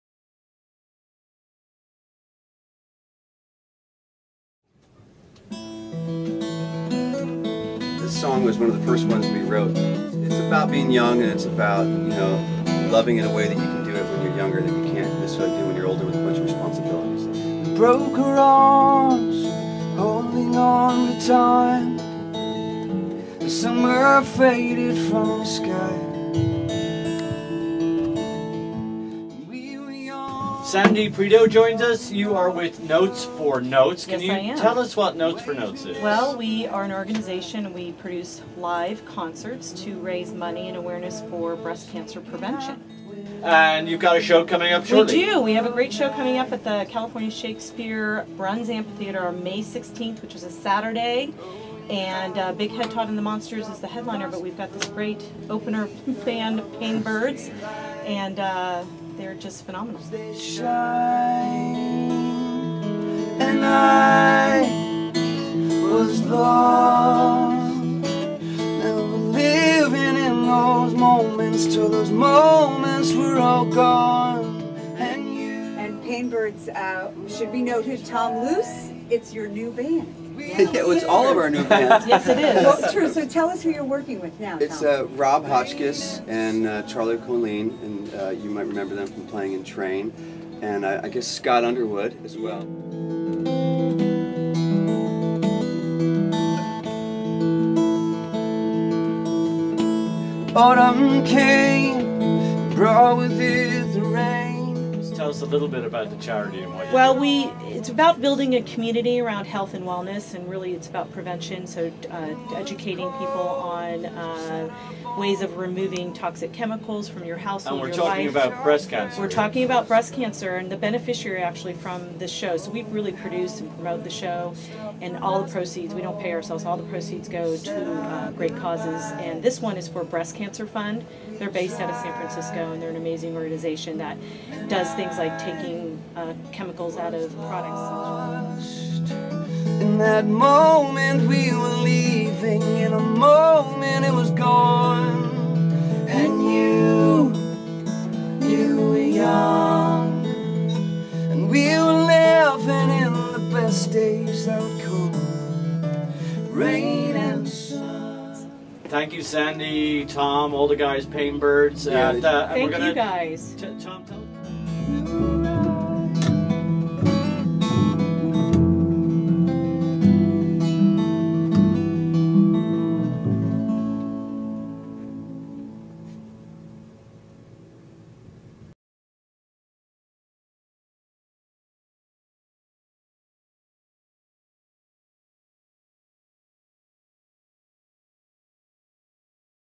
Notes4Notes.org on KFOG with paynbirds
Notes4Notes.org  KFOG interview talking with Sandy Preto and paynbirds (former train members Rob Hotchkiss, Charlie Colin and Tom Luce of LUCE) about the May 16th 2015, Breast Cancer prevention concert at Bruns Amphitheatre with Big Head Todd and the Monsters, paynbirds and special guest Megan Slankard.